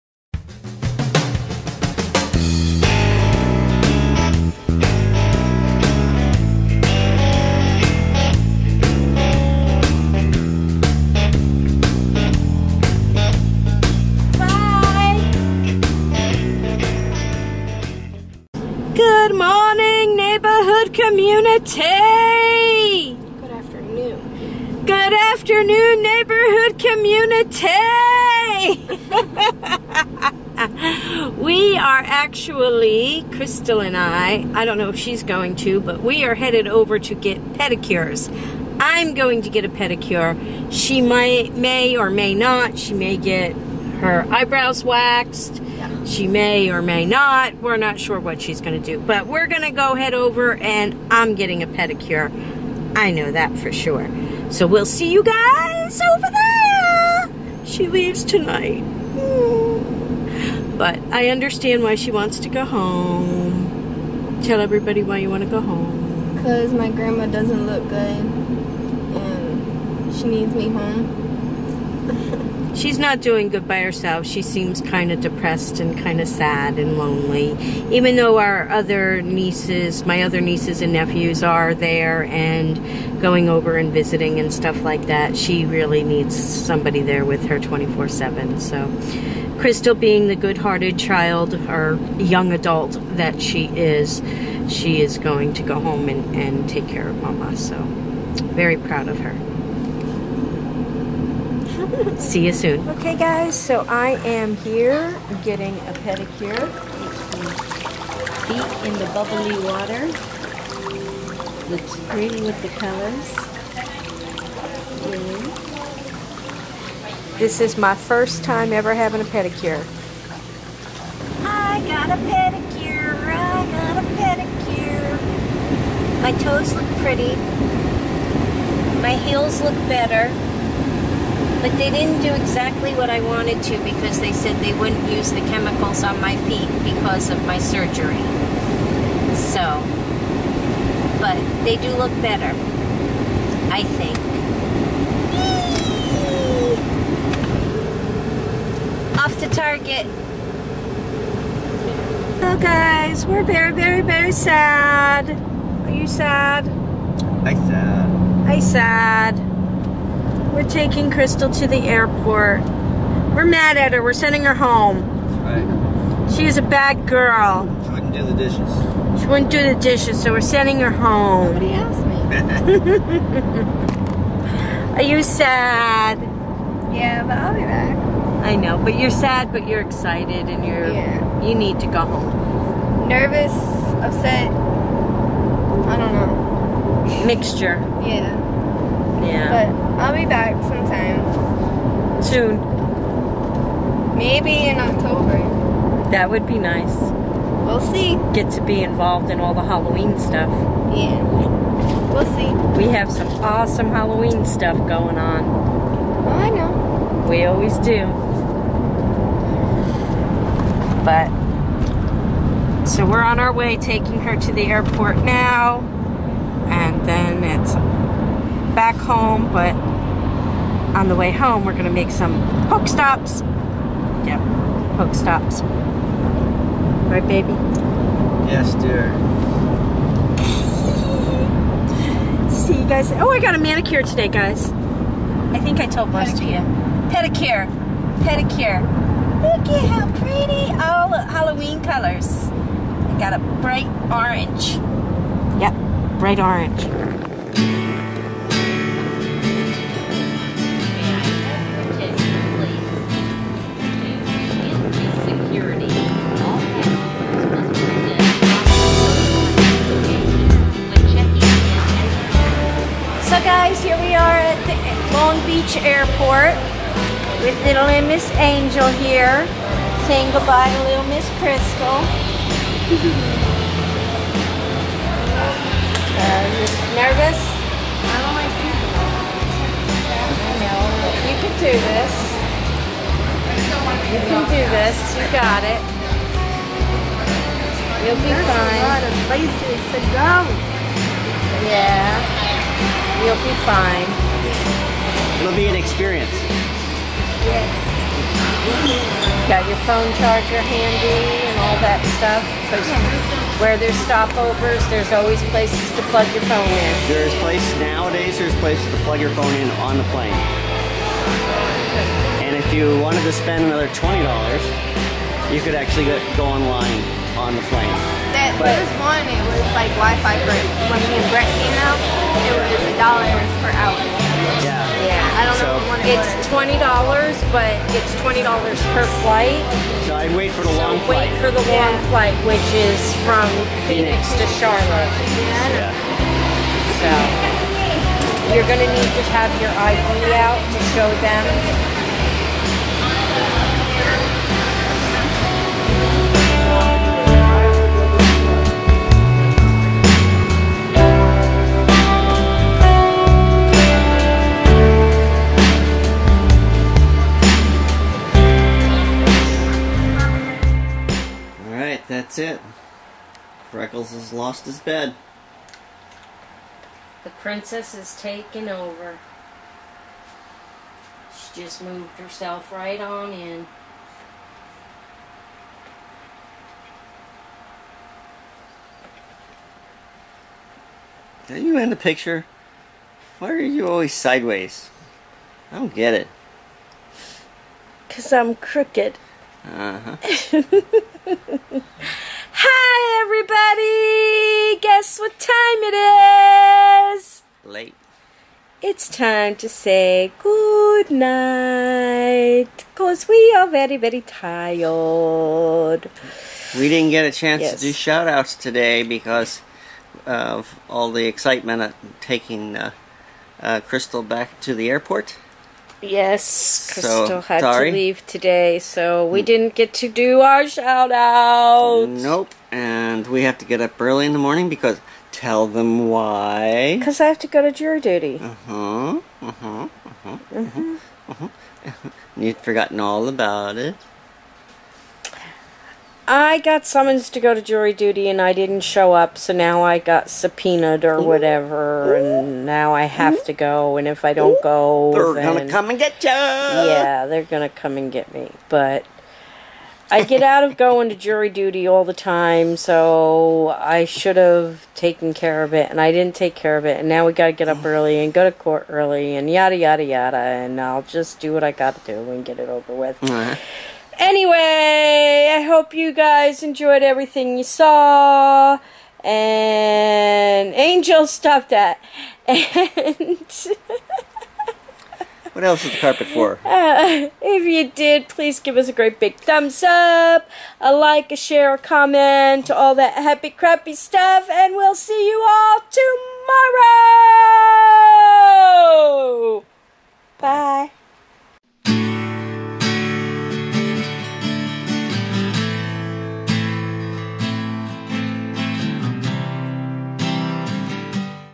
SAYING GOODBYE TO CHRYSTAL
Valerie and Chrystal go to get pedicures and eyebrow waxing done before Chrystal heads back to North Carolina! Kevin and Valerie take Chrystal to The Long Beach Airport for her long trip home! Gonna Miss Her!!

                    Kevin: 49drummers
                    Megan: meganceciliacoyne